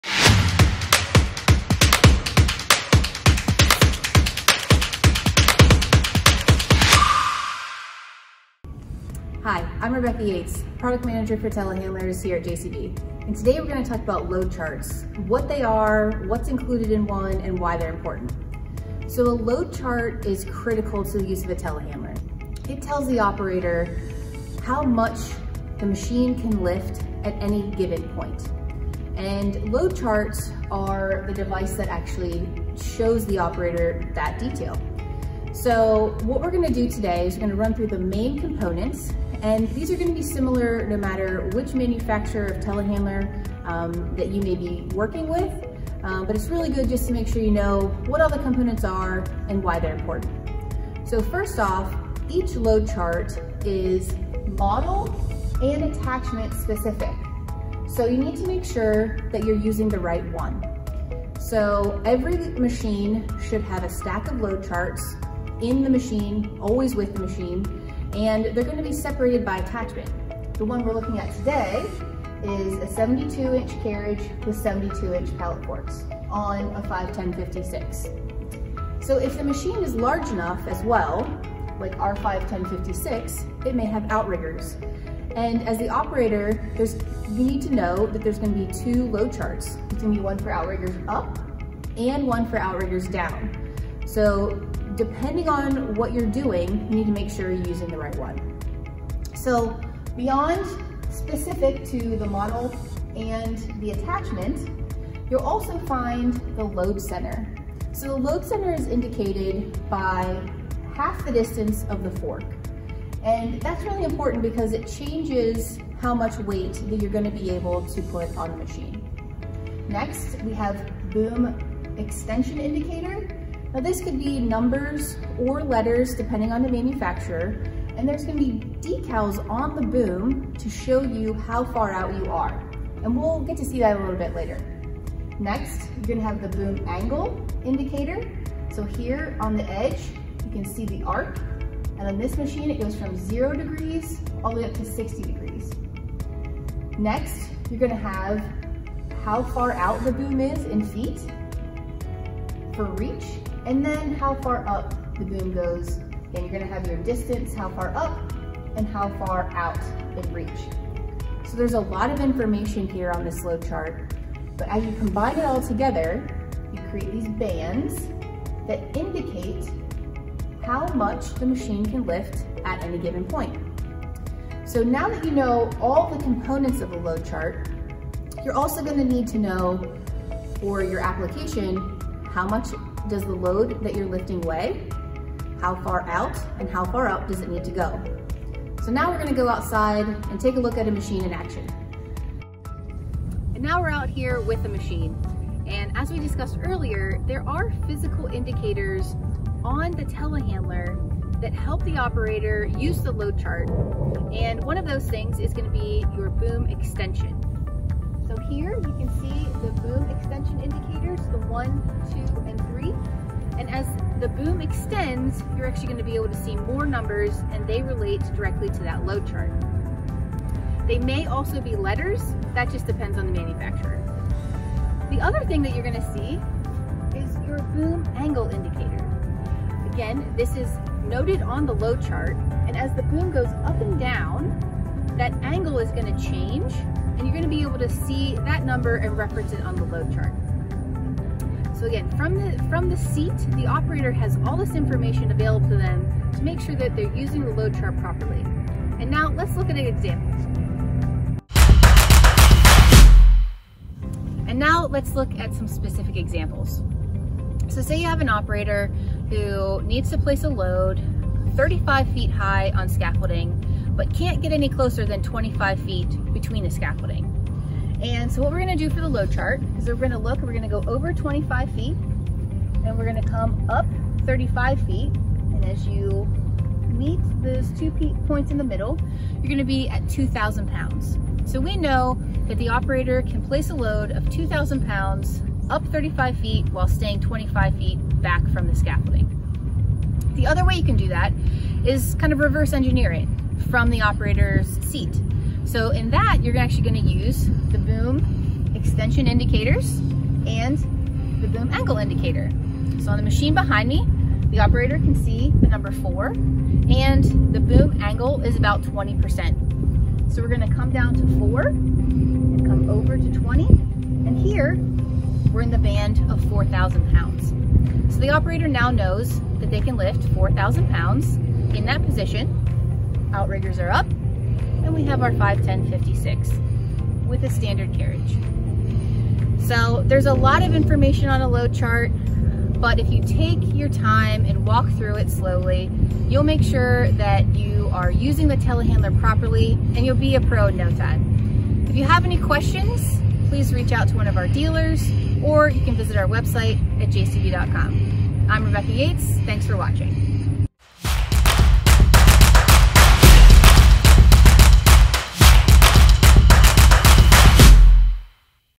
How to Read a Load Chart
Learn about the importance of a telehandler load chart, the components, and how the data shown combines to safely lift loads at any given point, as well as review of examples using both the material weight/placement and the boom position.